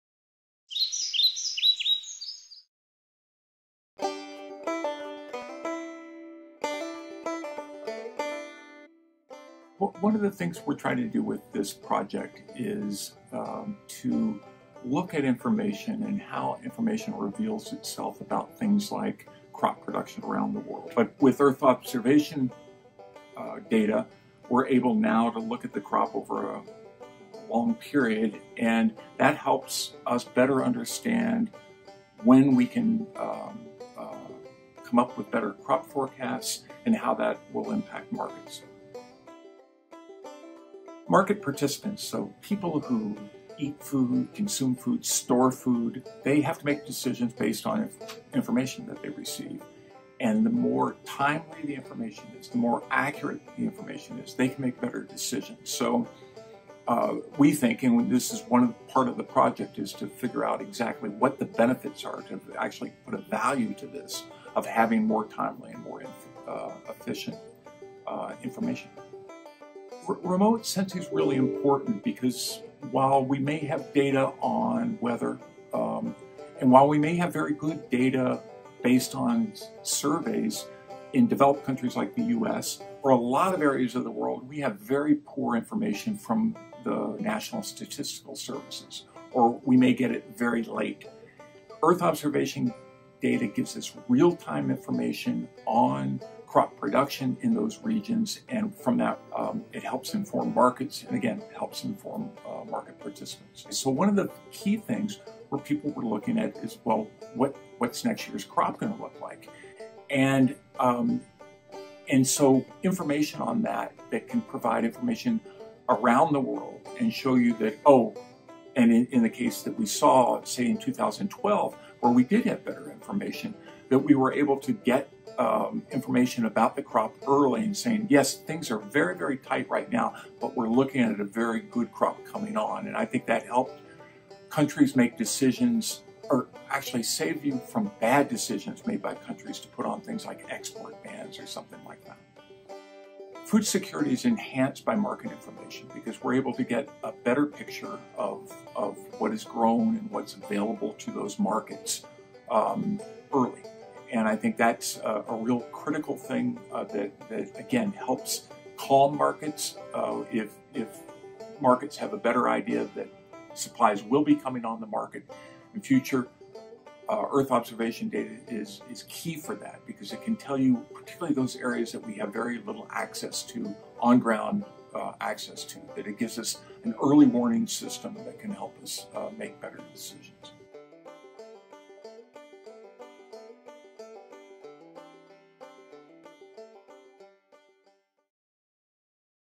Partner Feature: Joe Glauber on the Value of Satellite Data for Agriculture Markets
NASA Harvest Partner Feature: Joe Glauber Provides Perspective on the Value of Satellite Data for Agriculture Markets